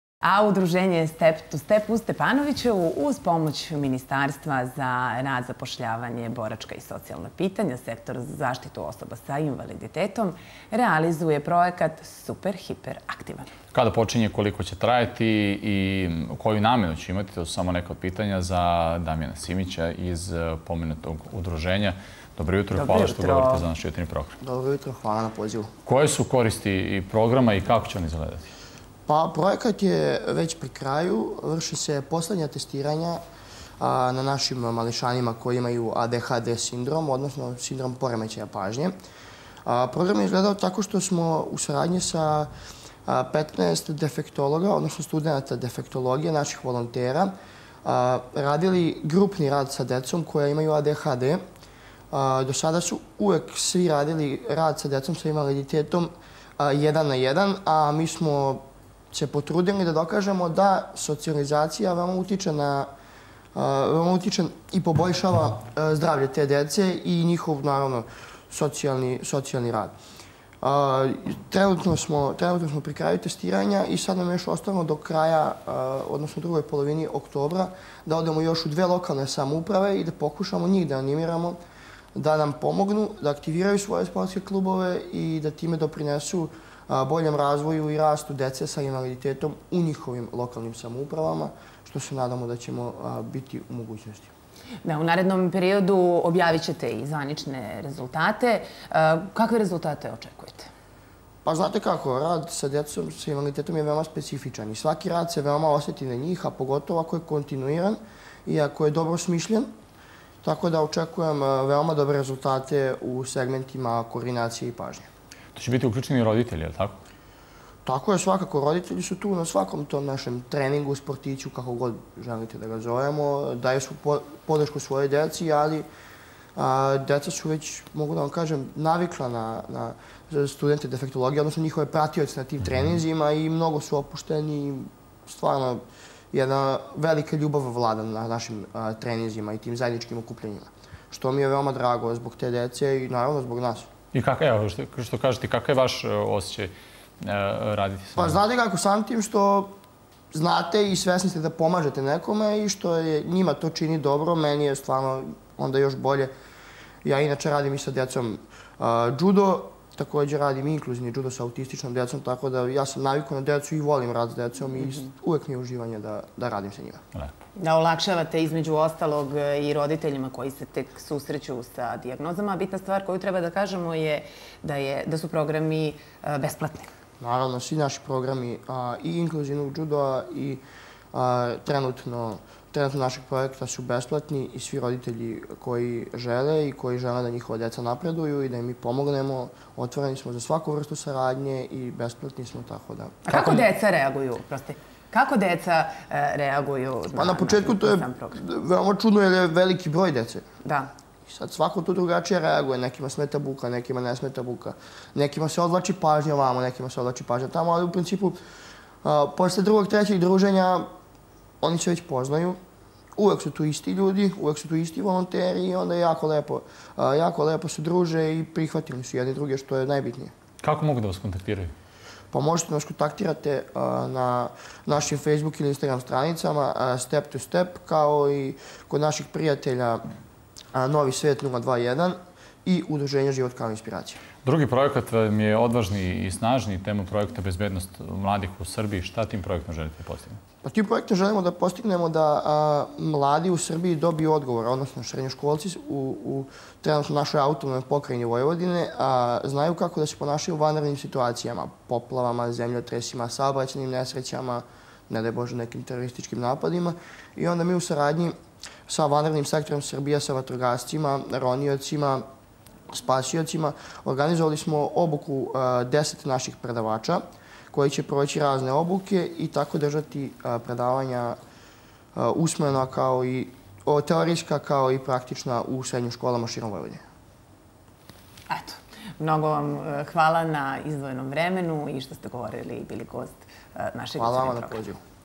Šta je projekat "Super-hiper-aktivan"?
Udruženje "Step to step" u Stepanovićevu, uz pomoć Ministarstva za rad, zapošljavanje, boračka i socijalna pitanja - Sektora za zaštitu osoba sa invaliditetom, realizuje projekat "Super-hiper-aktivan".  Gost Jutarnjeg programa bio je  Damjan Simić iz udruženja "Step to step" u Stepanovićevu.